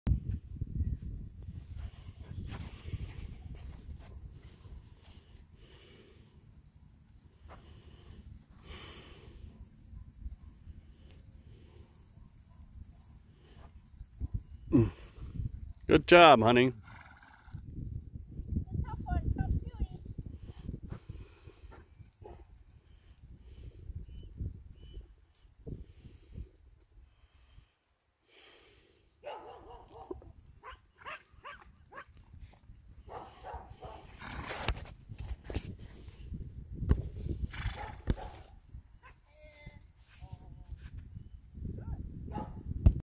New Paso friend for Ziggy
New Paso from Idaho. Very nice under saddle. Smooth and fast.